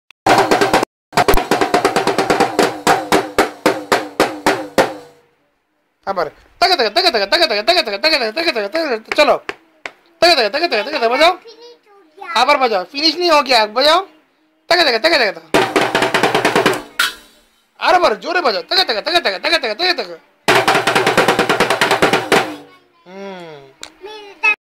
ADHIRAJ CHAUDHURI DRUMS
ADHIRAJ CHAUDHURI. ALSO KNOWN AS " KUSH ". ONLY 2 AND A HALF YEARS BABY BOY. PLAYING THE LITTLE TOY DRUM SET. A WONDER BOY AND A MUSICIAN IN THE MAKING.